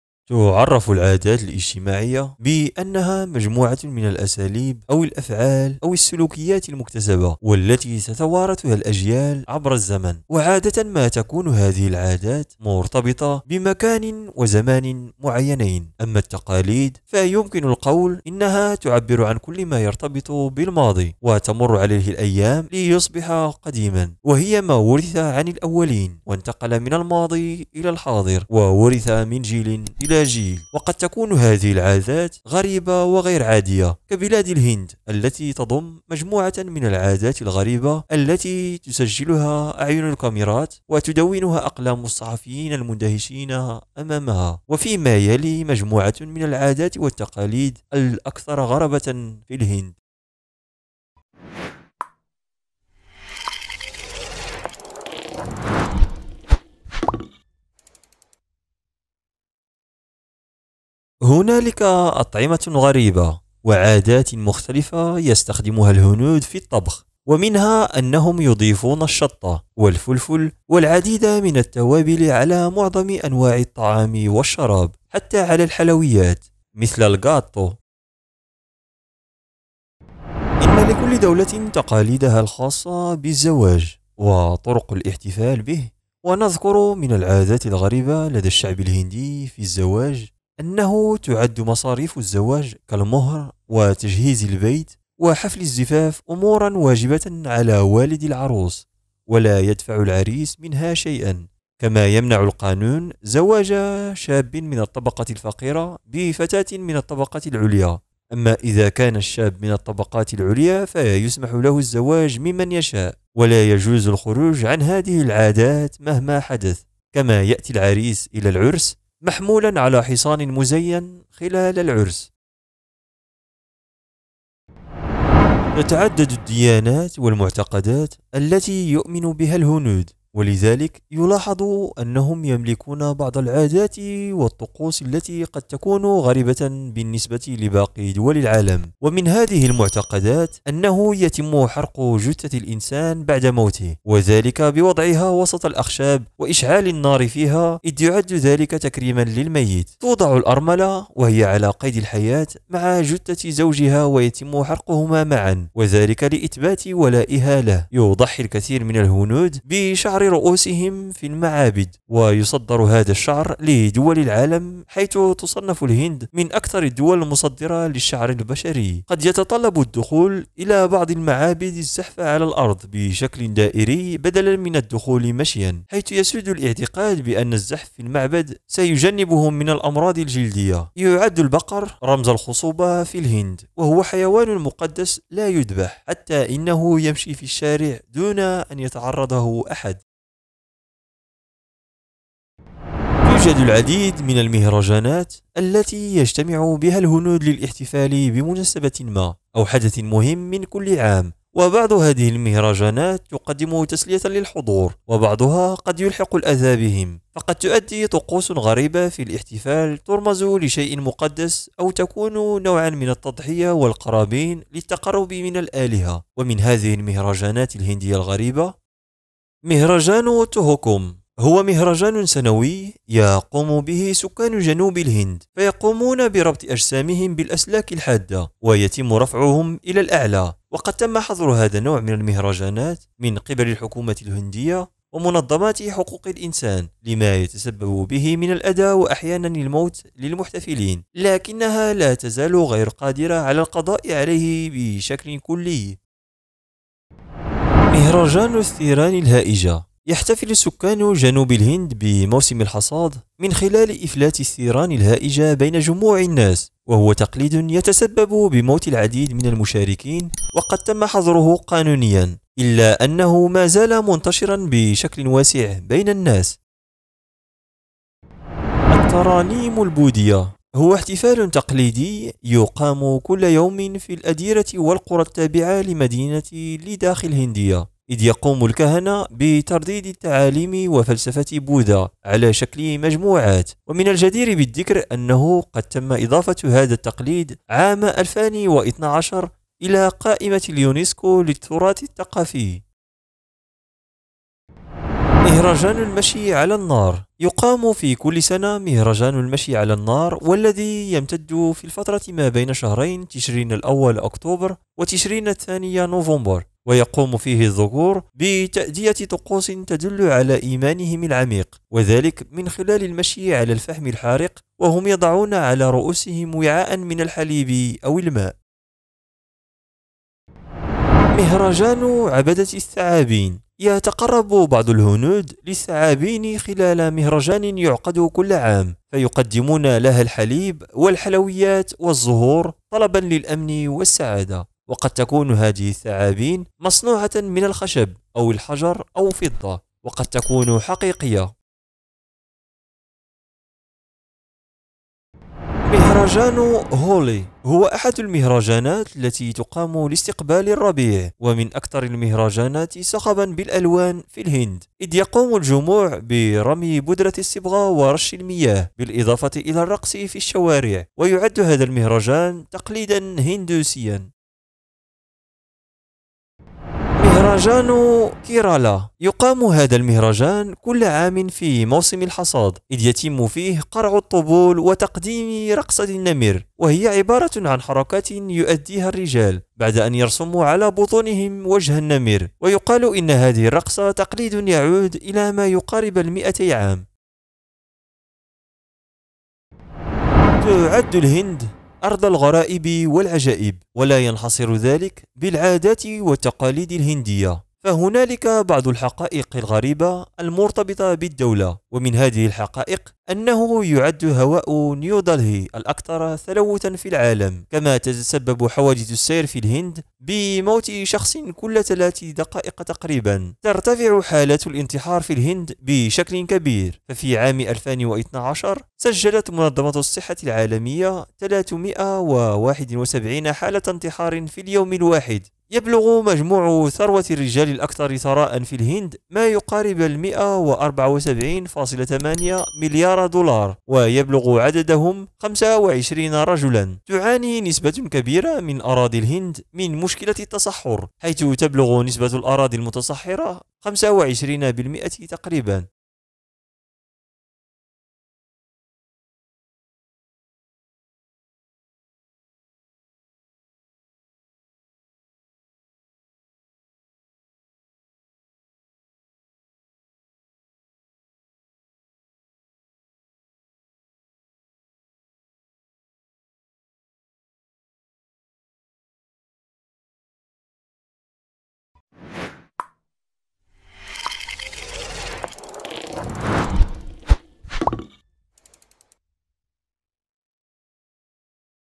غرائب وعجائب المجتمع الهندي...حقائق غريبة ومثيرة تعرفها لأول مرة عن الهند!!!
تقع الهند في جنوب قارّة آسيا، وتحدّها من الشمال الصين، ونيبال، وبوتان، وأفغانستان، ومن الجنوب سيرلانكا، أمّا من الغرب فتحدّها باكستان وبحر العرب، ومن الشرق خليج البنغال، وبنغلاديش، ومينمار. وتنقسم الهند إلى 28 ولاية، بالإضافة إلى سبع مناطق اتحاديّة، وتعد نيودلهي العاصمة الرسميّة للهند، أمّا أشهر المدن وأكبرها فهي مومباي، ودلهي، ومدارس، وحيدر آباد. وتعدّ الهند بمساحتها البالغة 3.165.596 كم2 سابعَ أكبر دولةٍ في العالم، كما تعدّ ثاني أكبر دولة من حيث عدد السكان بعد جمهوريّة الصين الشعبيّة، إذ يبلغ عدد سكانها ملياراً و263 مليوناً و344 ألفاً و631 نسمة حسب إحصائيات عام 2014. وتعدّ اللغتان الهنديّة والإنجليزيّة اللغتين الرسميتين في البلاد، بالإضافة إلى 41 لغة أخرى. كما يوجد عددٌ كبيرٌ من الدياناتِ إضافةً للهندوسية، والإسلامِ، والمسيحيّة، والبوذية، والسيخ..

00:00 المقدمة.
01:00 الطعام.
01:20 الزواج.
02:05 المعتقدات والطقوس.
03:13 مهرجانات غريبة في الهند.
03:39 مهرجان تهوكم.
04:07 مهرجان الثيران الهائجة.
04:28 الترانيم البوذيّة.
04:55 مهرجان المشي على النّار.
05:20 مهرجان عبادة الثعابين.
05:45 مهرجان هولي.
06:07 مهرجان كيرالا.
06:30 معلومات غريبة عن الهند.